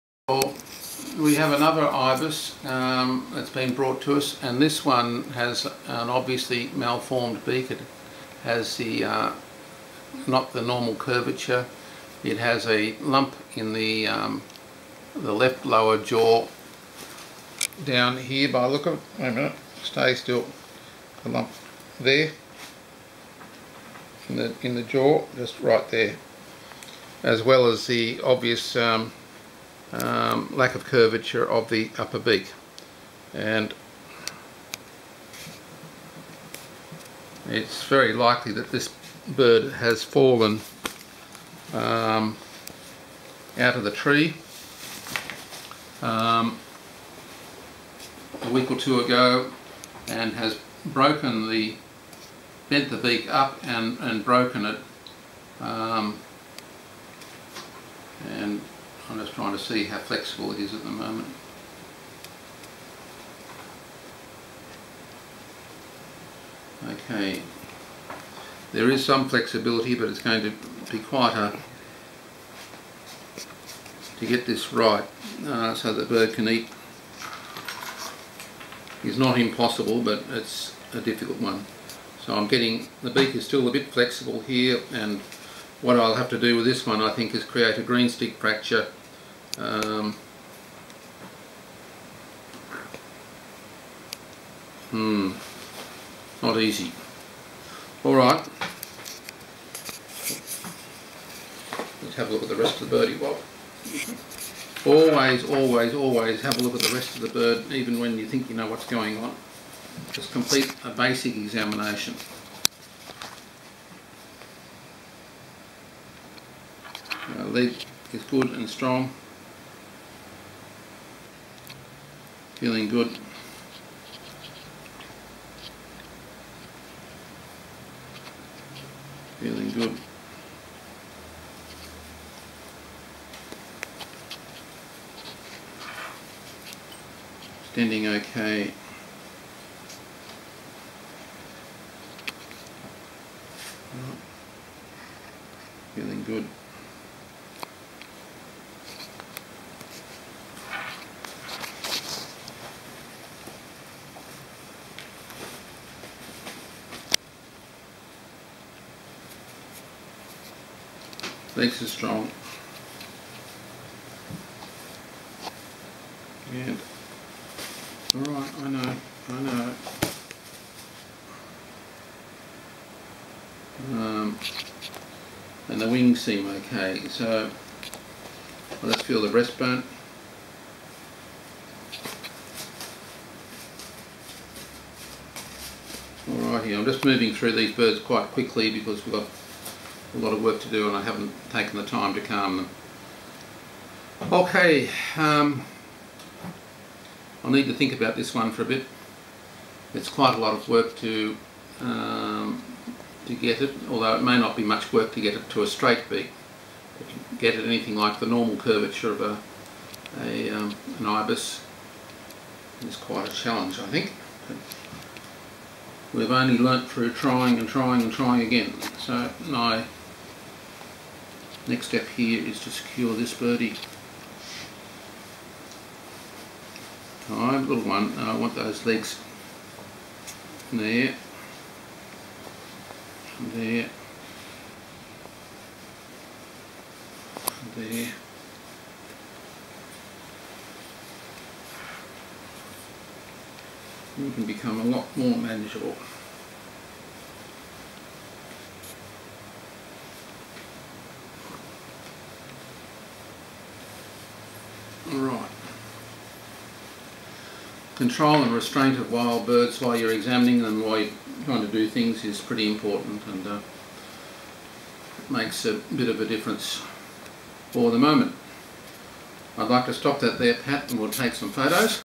Birds Ibis Chick with Very Deformed Beaks #1 120810
Bird Vet Dr Ross Perry brings you video clip #1 of a consultation for a wild Ibis chick with very deformed beaks rescued by Sydney Metropolitan Wildlife volunteers. Dr Ross Perry gives the chick a general physical check while he thinks about what might be tried to help this chick live a happy life.